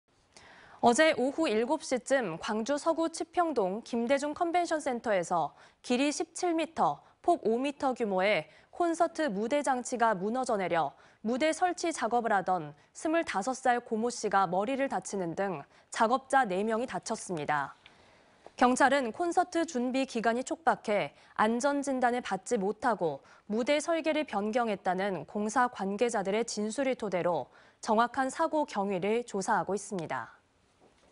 DJ센터서 콘서트 무대장치 무너져.. 작업자 4명 부상
어제(28) 오후 7시쯤,
광주 서구 치평동 김대중컨벤션센터에서
길이 17m, 폭 5m 규모의
콘서트 무대장치가 무너져 내려
무대 설치하던 작업을 하던
25살 고 모 씨가 머리를 다치는 등
작업자 4명이 다쳤습니다.

경찰은 "콘서트 준비 기간이 촉박해
안전 진단을 받지 못하고 무대 설계를 변경했다"는
공사 관계자들의 진술을 토대로
정확한 경위를 조사하고 있습니다.